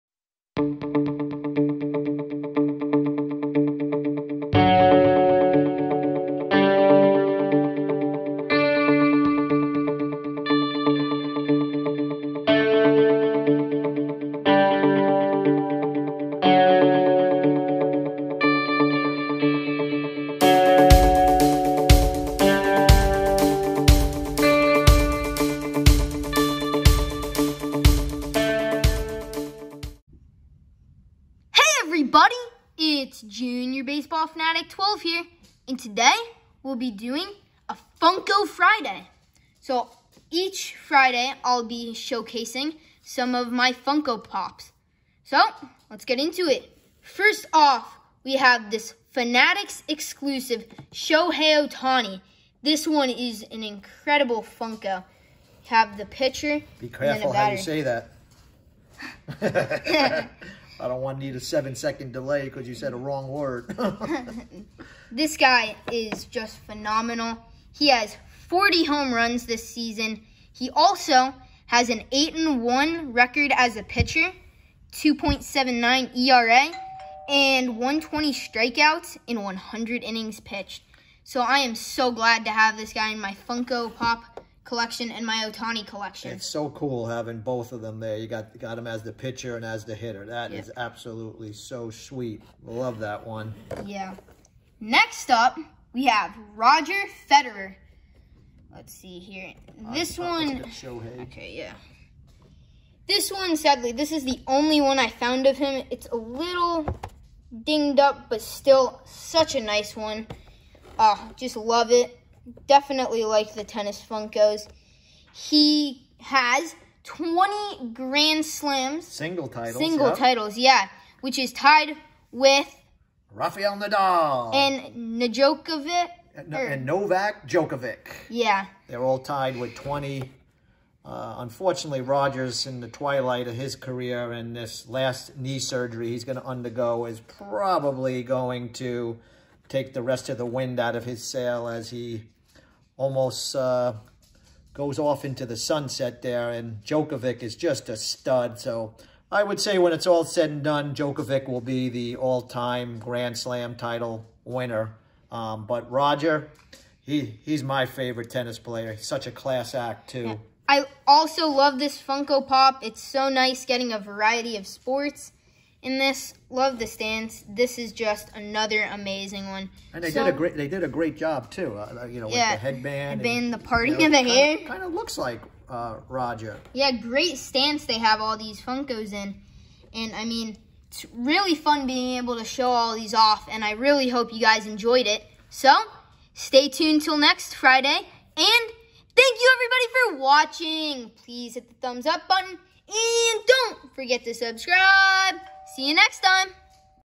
Funko Friday: Me & dad showing off some of my Funko pops.
Funko Friday: Me & dad showing off some of my Funko pops. Hope you enjoy and hope Rocky (Rad & Dad & B-dog) enjoys the first one.